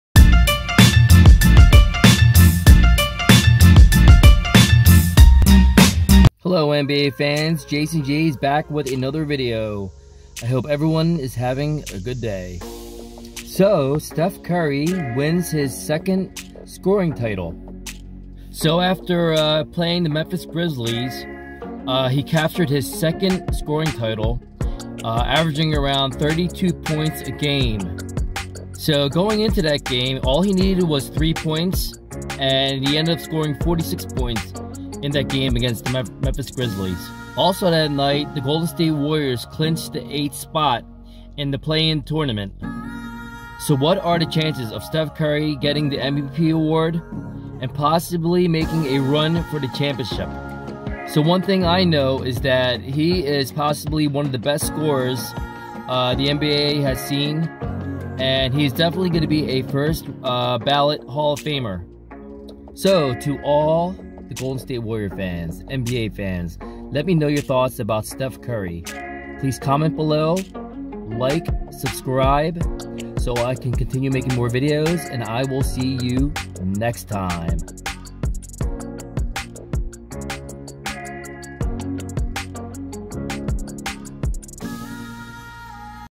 STEPHEN CURRY gets NBA Scoring Title! Golden State Warriors News - Stephen Curry warriors
STEPHEN CURRY of the Golden State Warriors wins his 2nd Scoring Title! Stephen Curry warriors